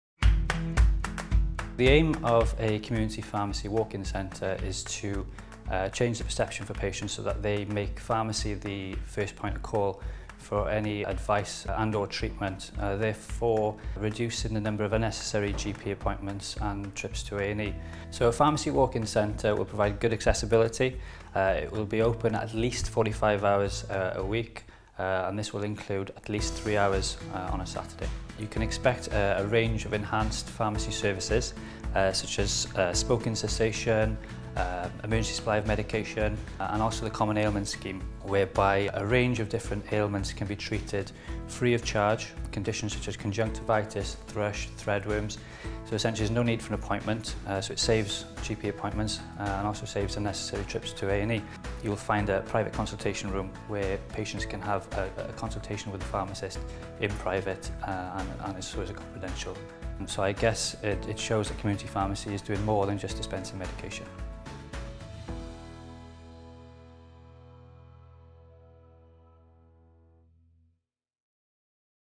Ed John, Clunderwen Pharmacy Walk-in Centre
A number of community pharmacies across Carmarthenshire, Ceredigion and Pembrokeshire are now known as Pharmacy Walk-in Centres since St David’s Day, 1 March 2019.

The participating pharmacies, who already offer a range of services for the public, are now being formally recognised under this scheme.

To see where your nearest Pharmacy Walk-in Centre is and to find out more about the services your local pharmacy offers, please